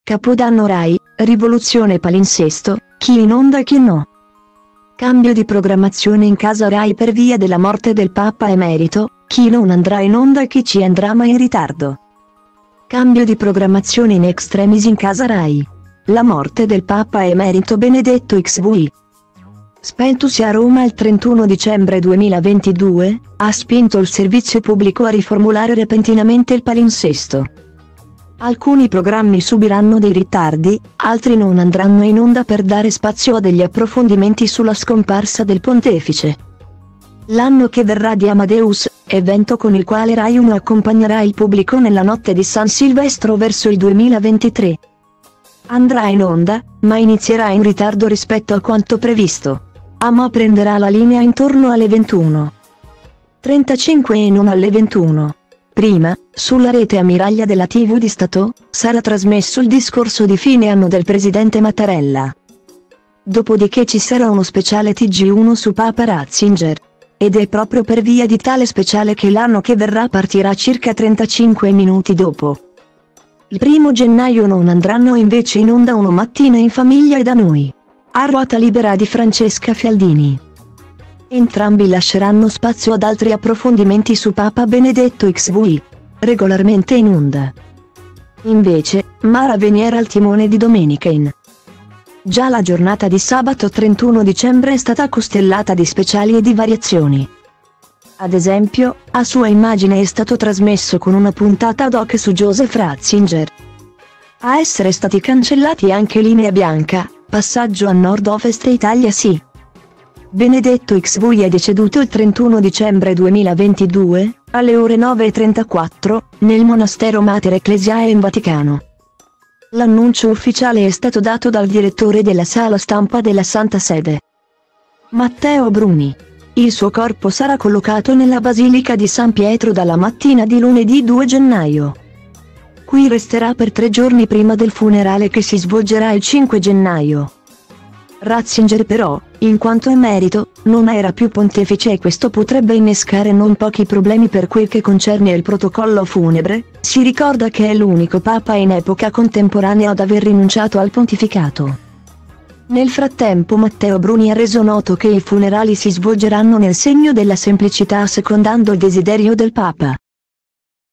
Capodanno Rai, rivoluzione palinsesto: chi in onda e chi no
Cambio di programmazione in casa Rai per via della morte del Papa emerito: chi non andrà in onda e chi ci andrà ma in ritardoCambio di programmazione in extremis in casa Rai. La morte del Papa emerito Benedetto XVI,spentosi a Roma il 31 dicembre 2022, ha spinto il servizio pubblico a riformulare repentinamente il palinsesto.